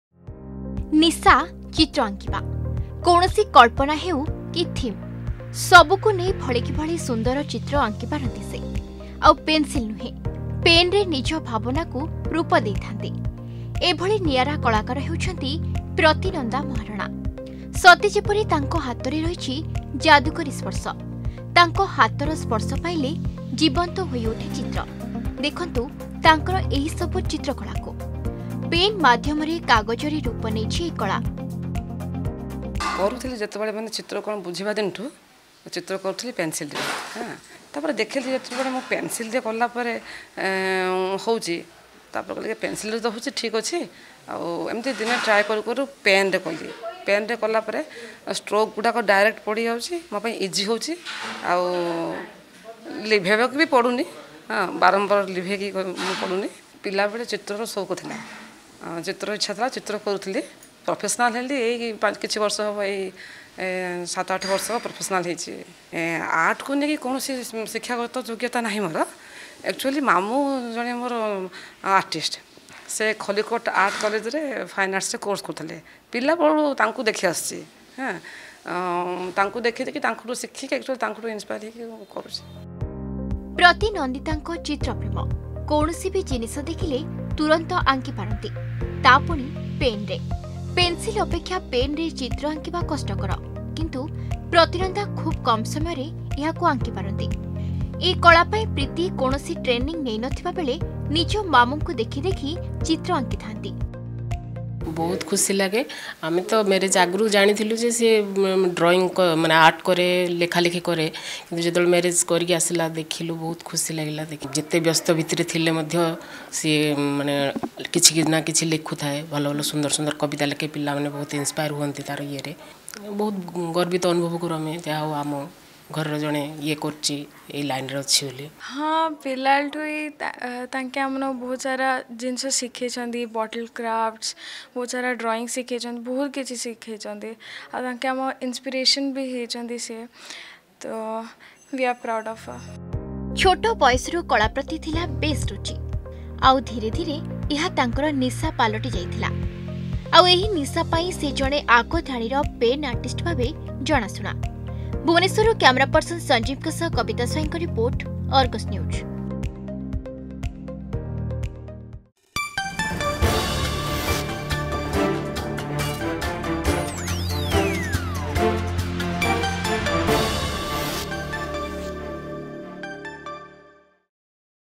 ବଲ ପେନରେ ଚିତ୍ରକଳା
ବଲ ପେନରେ ଚିତ୍ରକଳା । ଭଳିକି ଭଳି ନିଆରା କଳାକୃତି । ସତେ ଯେପରି କଥା କହୁଛି ଚିତ୍ର । କୌଣସି ବି ଥିମକୁ ନେଇ ଚିତ୍ର ଆଙ୍କିଥାନ୍ତି ପ୍ରତିନନ୍ଦା ମହାରଣା.